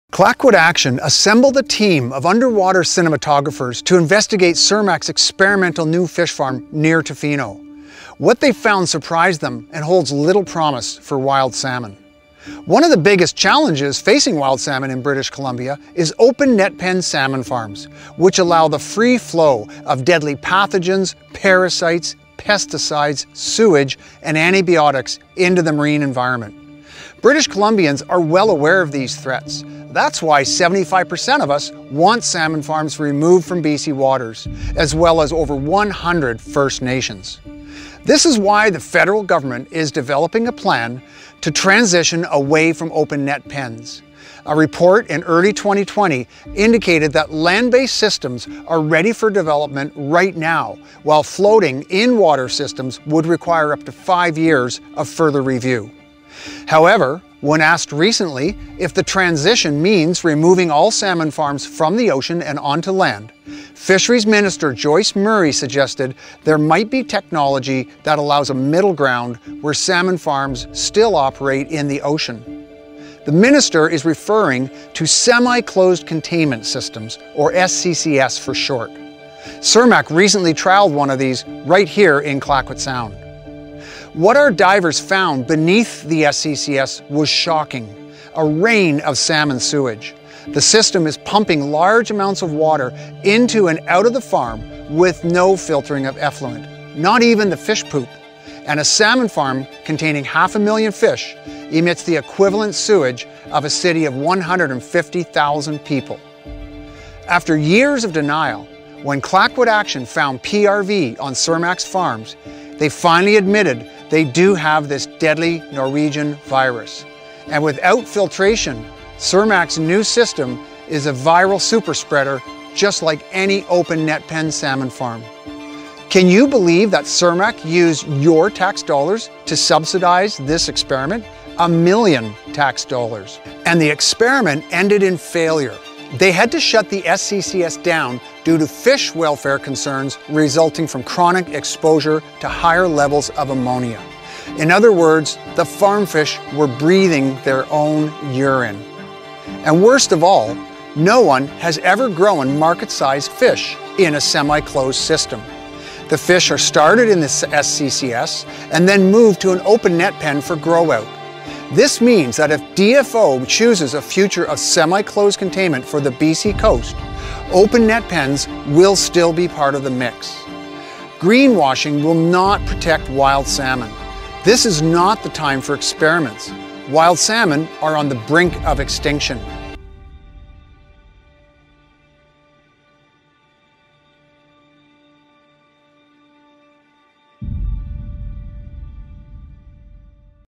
Semi-closed containment systems will not protect wild salmon | Wild First
The science is clear, band-aid solutions won’t work.

👉Read more on why salmon farms have to come out of the water from Living Oceans Society

Video Credits: Clayoquot Action & Jérémy Mathieu